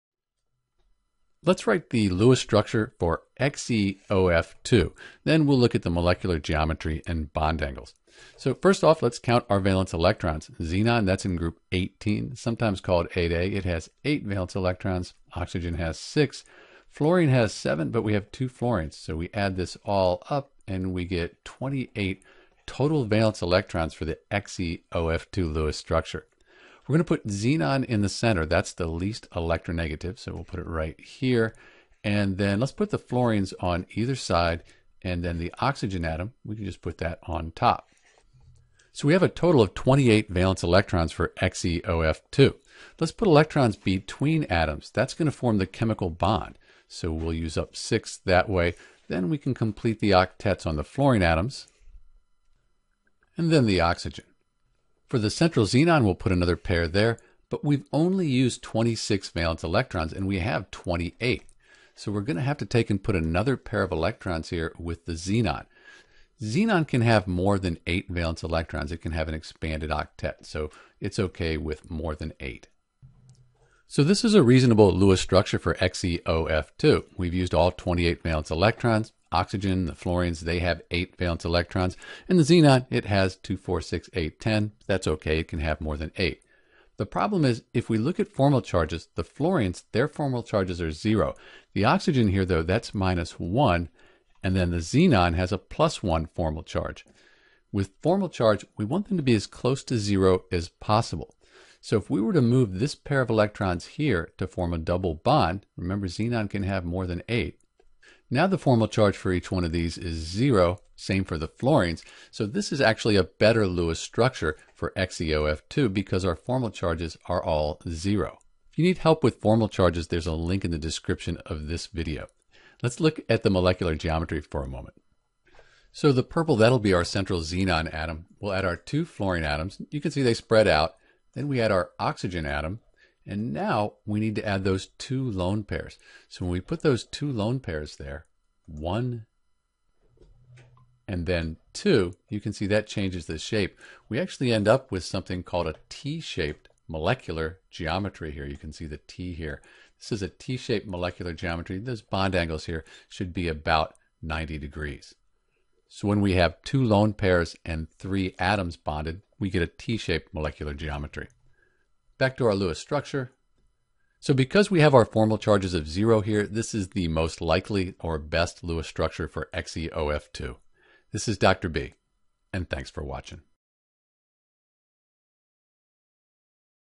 Lewis Dot Structure for XeOF2 (and Molecular Geometry)
A step-by-step explanation of how to draw the XeOF2 Lewis Dot Structure (Xenon oxydifluoride).

For the XeOF2 structure use the periodic table to find the total number of valence electrons for the XeOF2 molecule. Once we know how many valence electrons there are in XeOF2 we can distribute them around the central atom with the goal of filling the outer shells of each atom.

In the Lewis structure of XeOF2 structure there are a total of 28 valence electrons. XeOF2 is also called Xenon oxydifluoride.

Steps to Write Lewis Structure for compounds like XeOF2
1. Find the total valence electrons for the XeOF2 molecule.
2. Put the least electronegative atom in the center. Note: Hydrogen (H) always goes outside.
3. Put two electrons between atoms to form a chemical bond.
4. Complete octets on outside atoms.
5. If central atom does not have an octet, move electrons from outer atoms to form double or triple bonds.

Lewis Structures, also called Electron Dot Structures, are important to learn because they help us understand how atoms and electrons are arranged in a molecule, such as Xenon oxydifluoride. This can help us determine the molecular geometry, how the molecule might react with other molecules, and some of the physical properties of the molecule (like boiling point and surface tension).